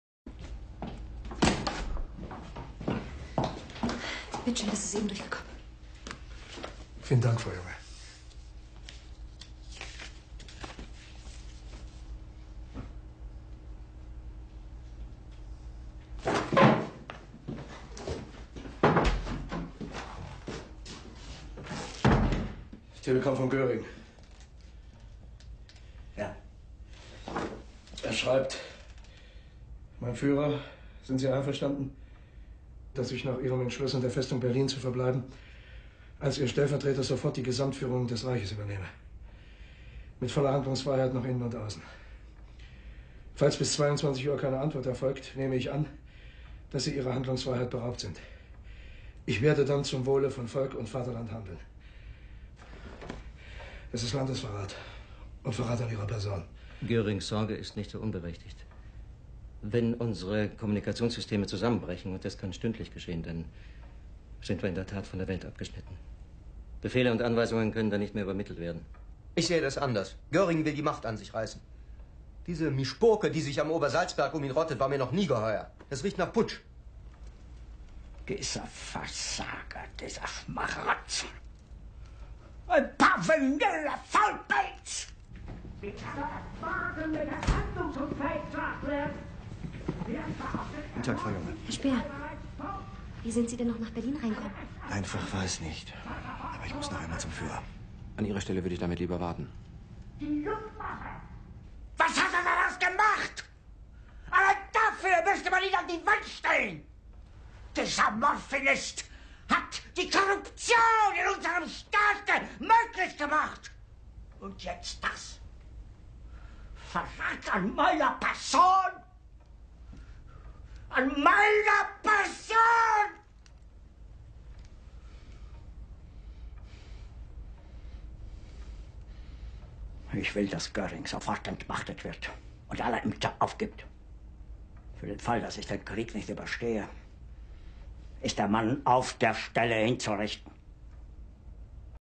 Hitler Snow-Day II
Hitler find out again that Campbell Savona has kept school open.